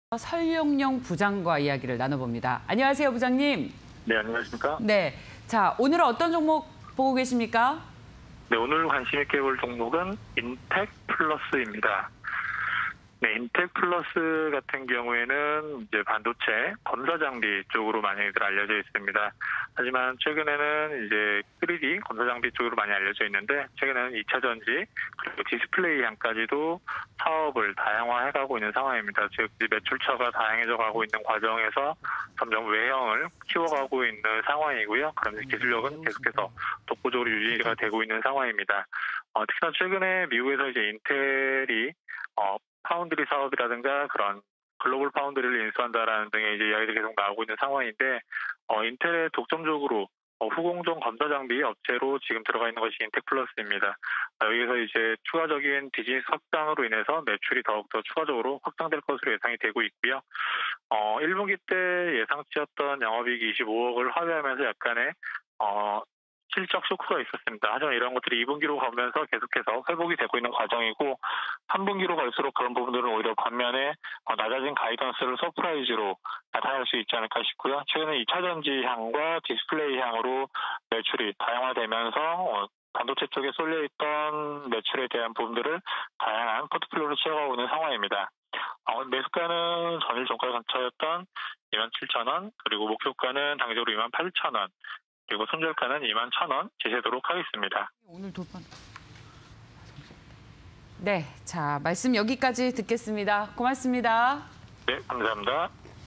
인텍플러스(064290)... 머신비전 기반 3D 검사장비 개발·판매_굿머니 공략주 (20210721)
인텍플러스(064290)
- 머신비전 기반 3D 검사장비 개발·판매
- 다양한 분야로 사업 확장성 보유
- 2분기 실적 회복세 예상